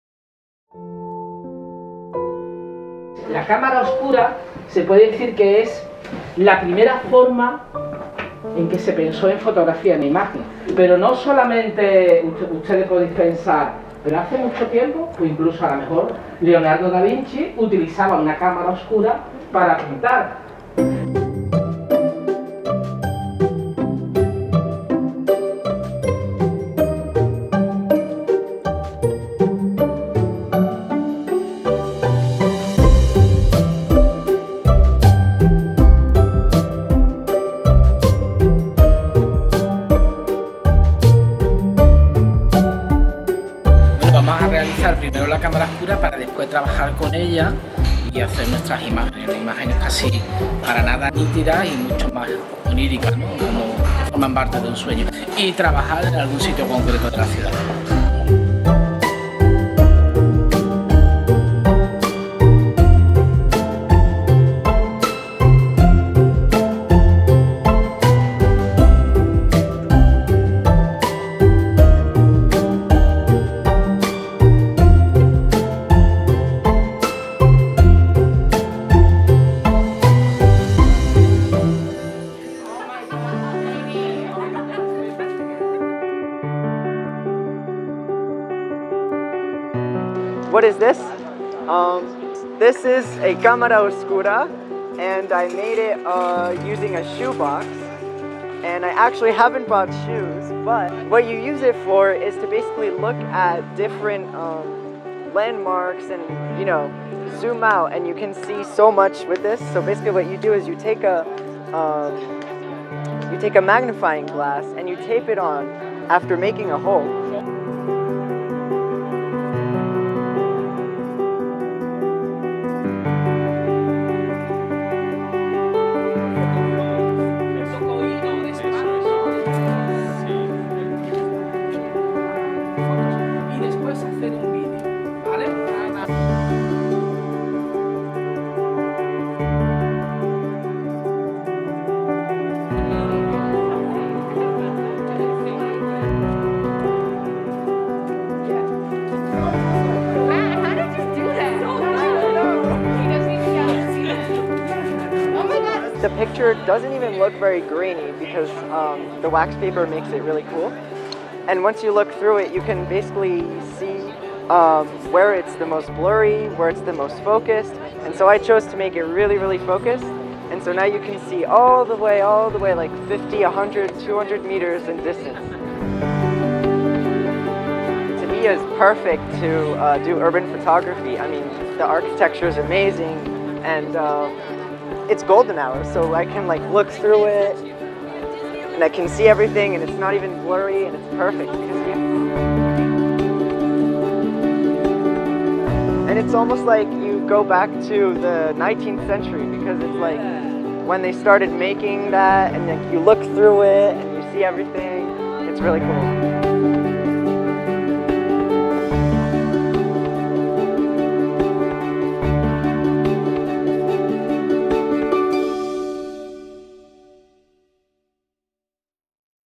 Verto Education Urban Photography Workshop in Seville, Spain - Fall 2021
The photographer Antonio Perez facilitates a workshop in which the students build a "camera obscura" using shoe boxes and magnifying glasses. This rudimentary instrument shows them the optical principles of photography. The fun comes when the cameras are put to use in the streets of Seville.